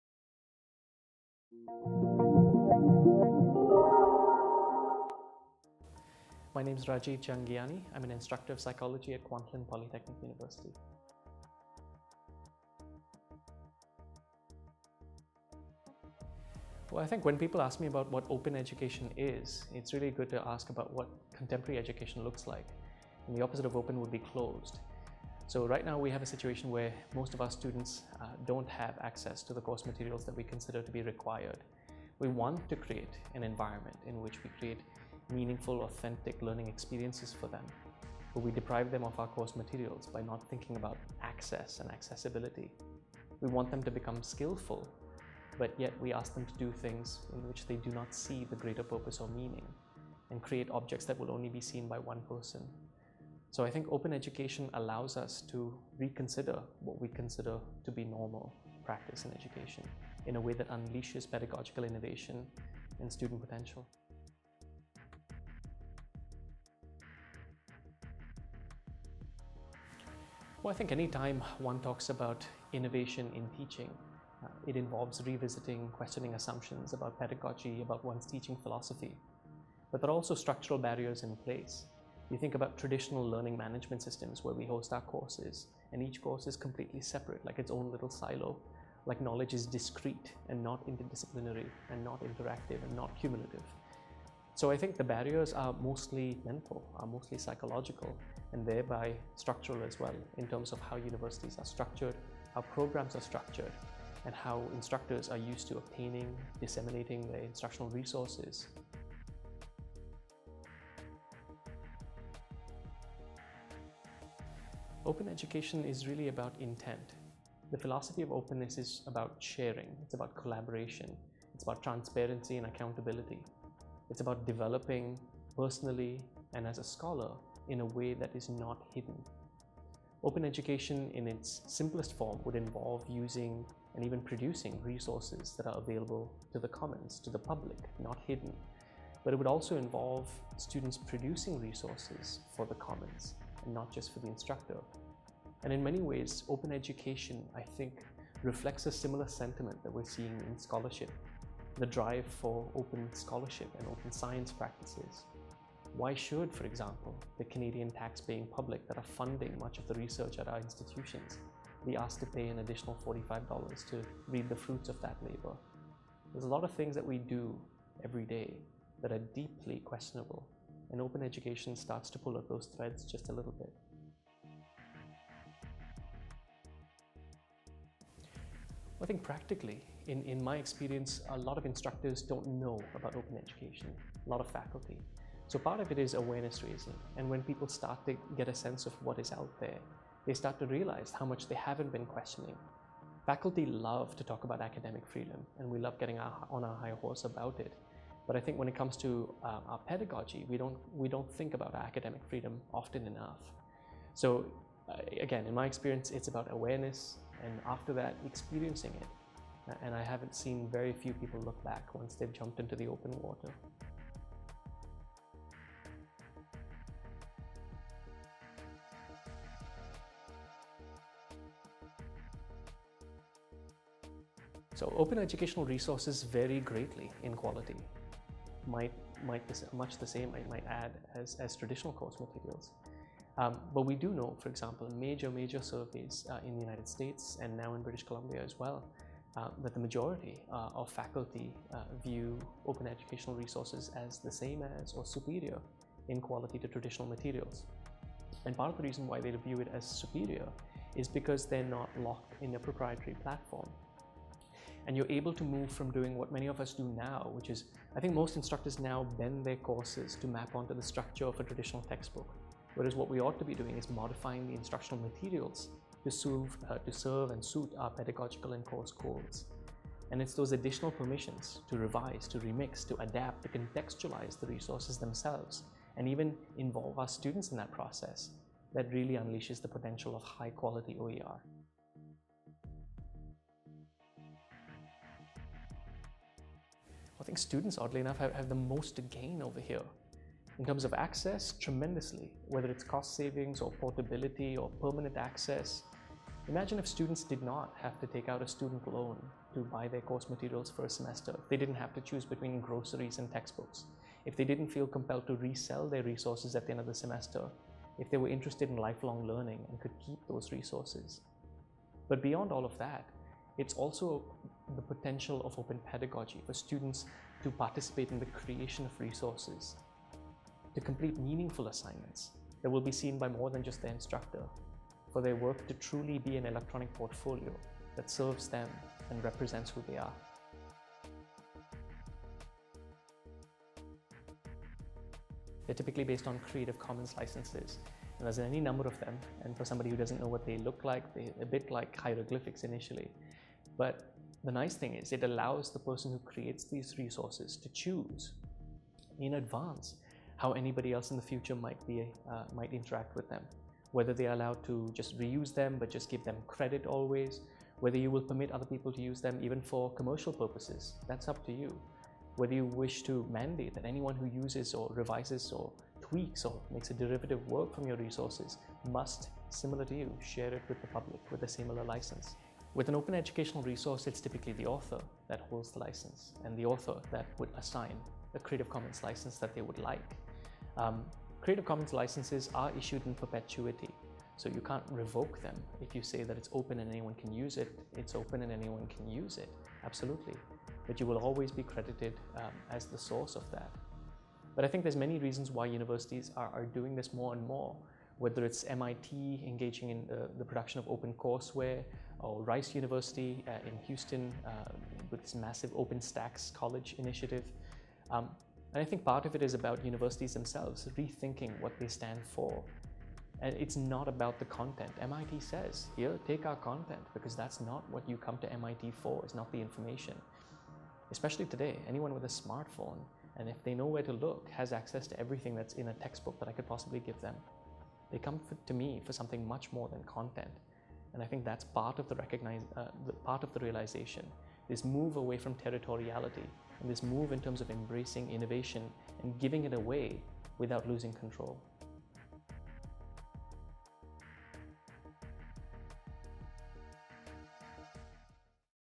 Interview with Rajiv Jhangiani on 'Enhancing Pedagogy via Open Educational Practices'
February 9th, 2016 at McMaster Institute for Innovation and Excellence in Teaching and Learning, McMaster University

“Open educational practices” is a broad term that encompasses the creation and adoption of open textbooks and other open educational resources, open course development, and open pedagogy. This presentation will make a case for why the move away from traditional (closed) practices is not only desirable but inevitable, and how students, faculty, institutions, and our communities all stand to benefit greatly from this transformation..

Rajiv Jhangiani is a faculty member in the Department of Psychology at Kwantlen Polytechnic University. He conducts research on open education, the scholarship of teaching and learning, and political psychology.